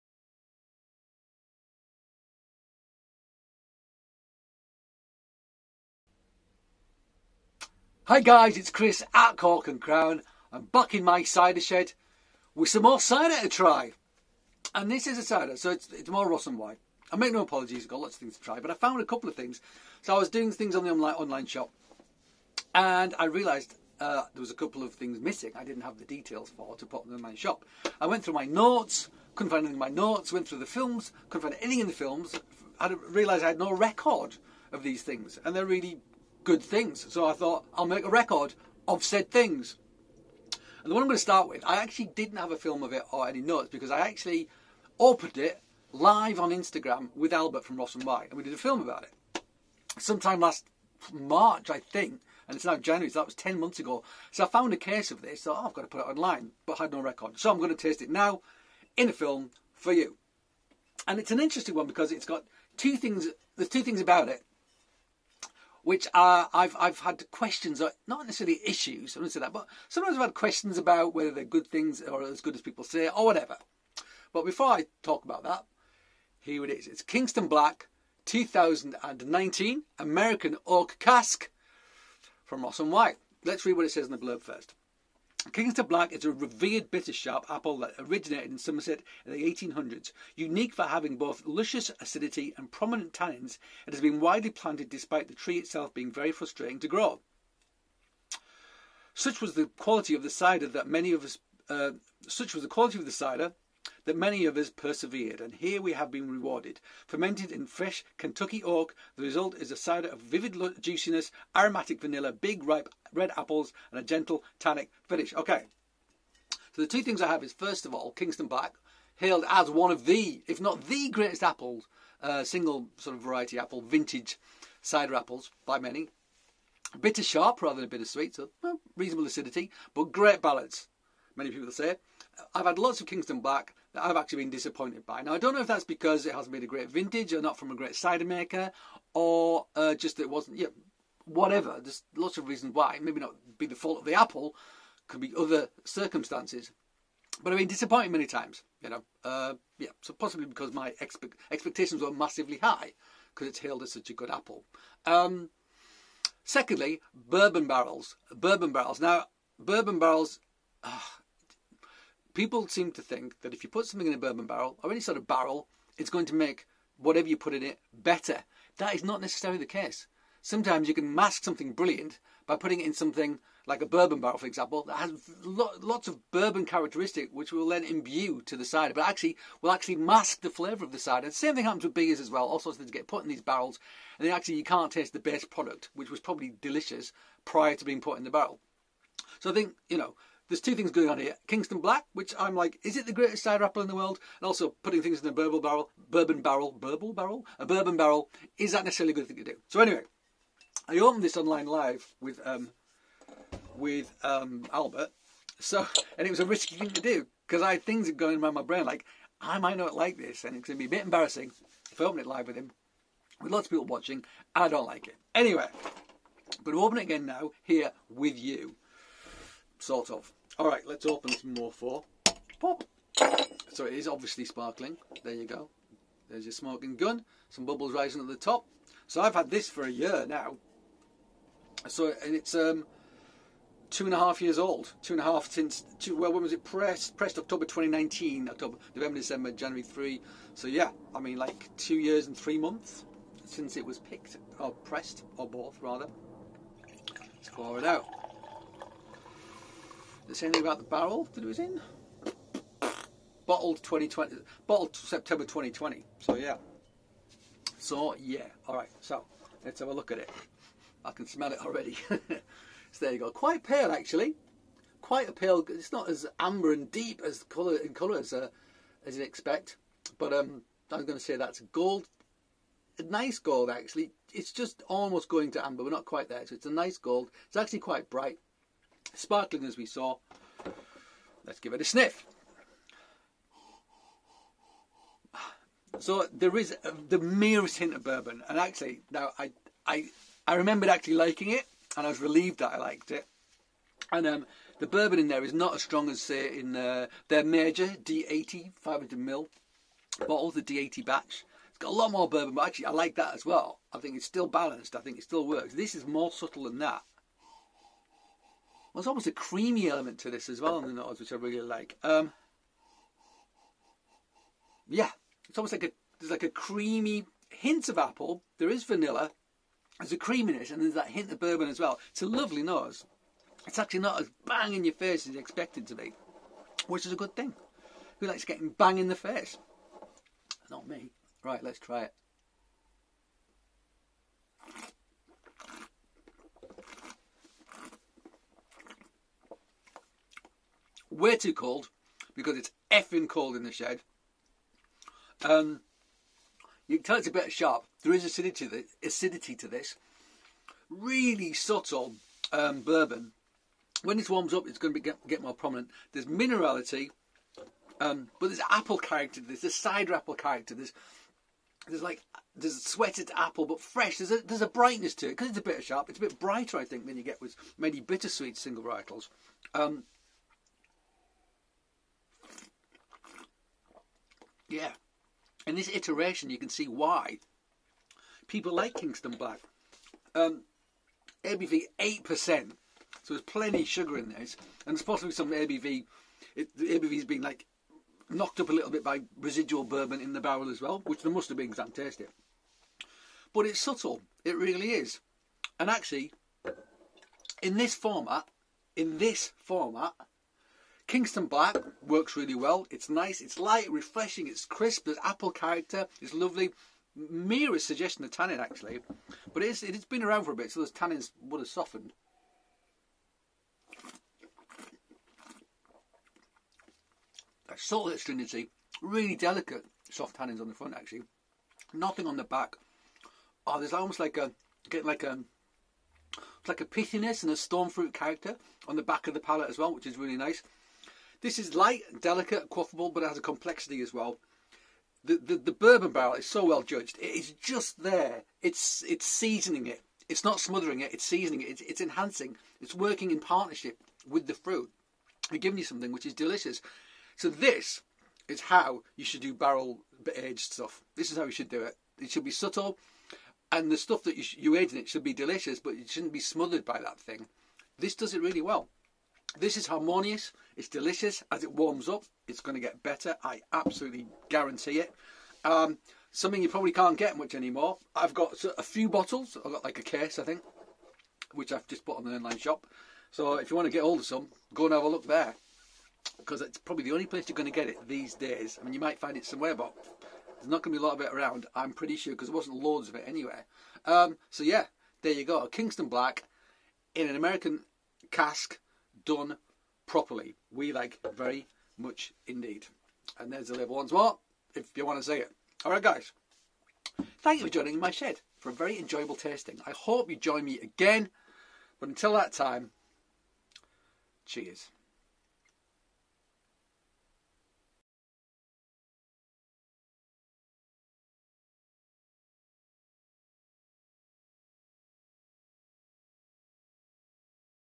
#381 Ross on Wye 'Kingston Black' American Whisky Cask 2019
Chris first tried this cider a very long time ago under a certain amount of scrutiny. He tries it by himself this time.